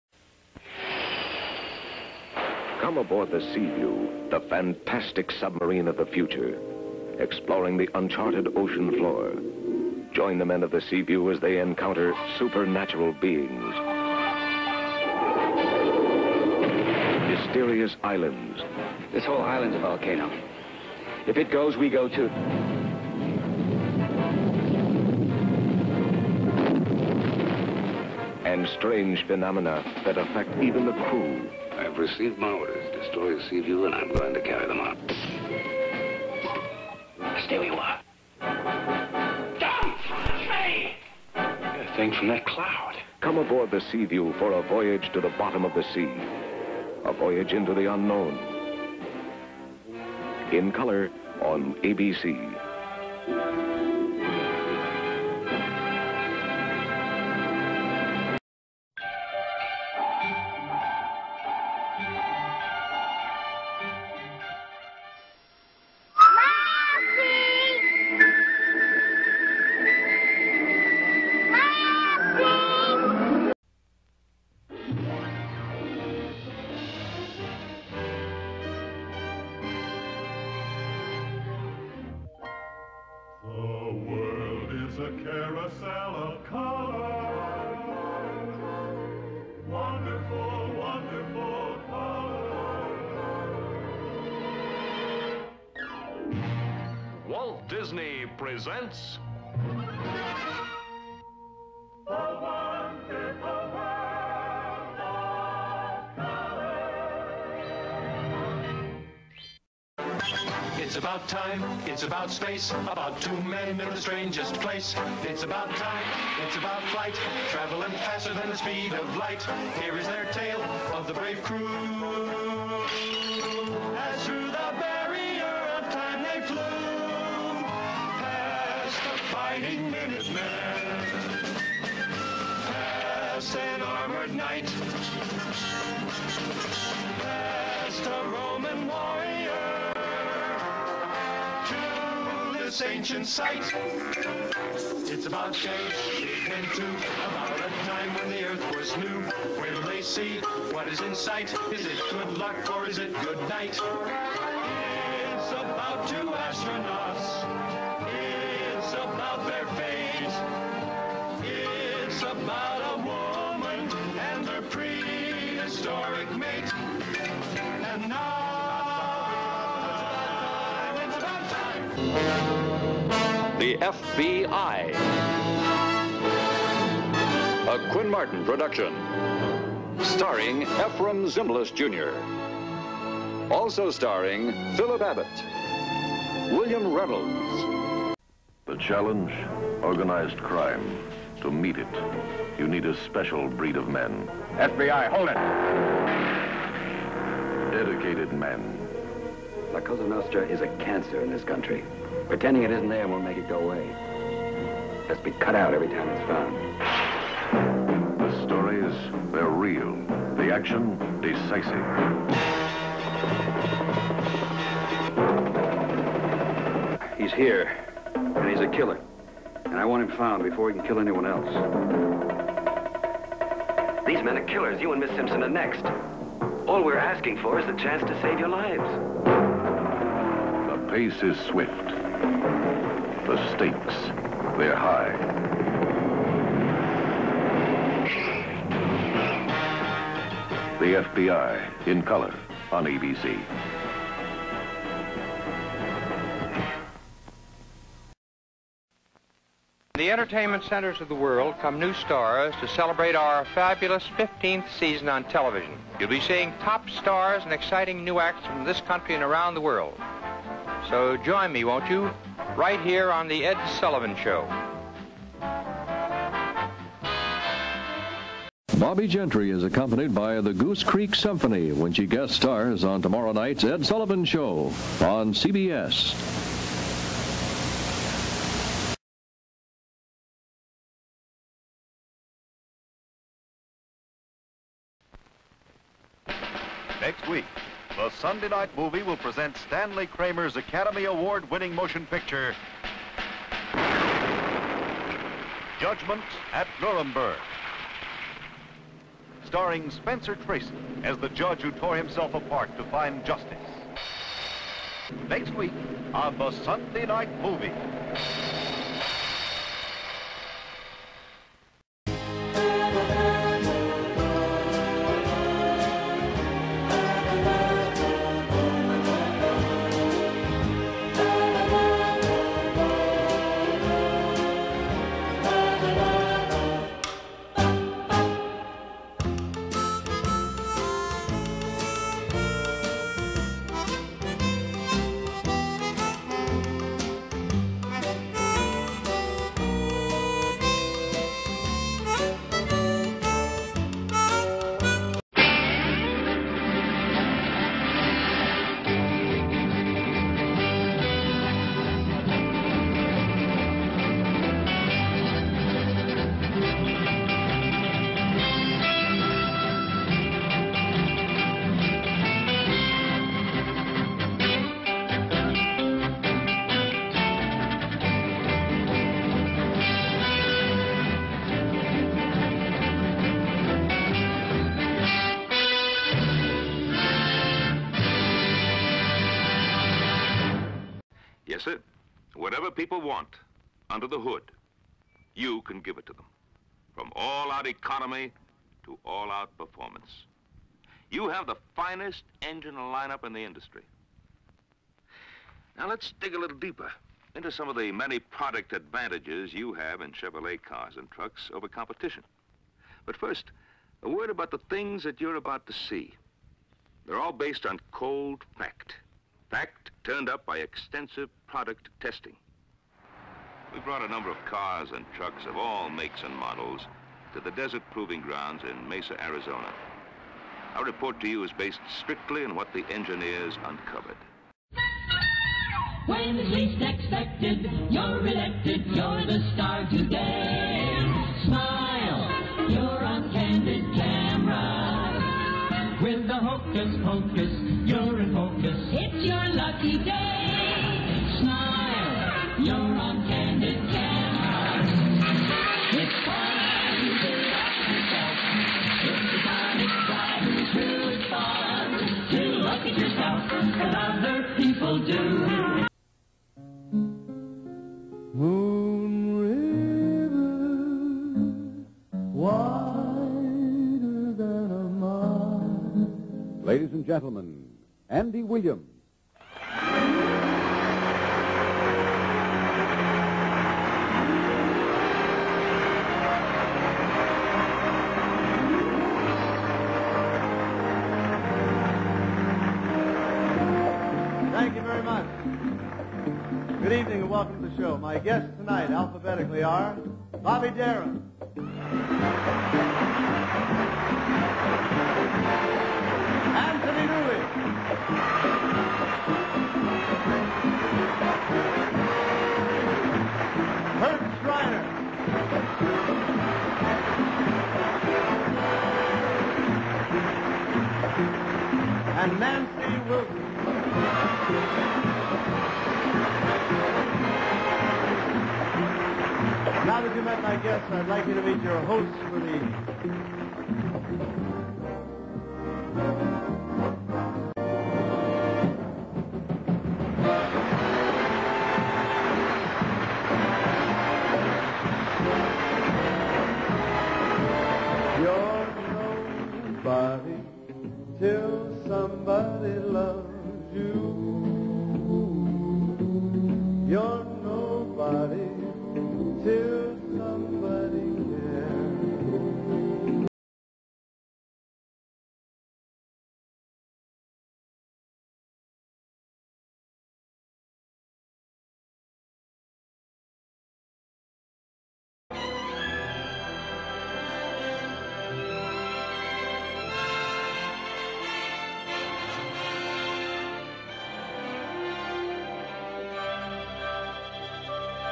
Sunday Night TV 1966-67 Season w/rare promos and commercials (The Best of FredFlix)
This early video was at the bottom of my channel. I'm re-posting it so that my newer subscribers can see it.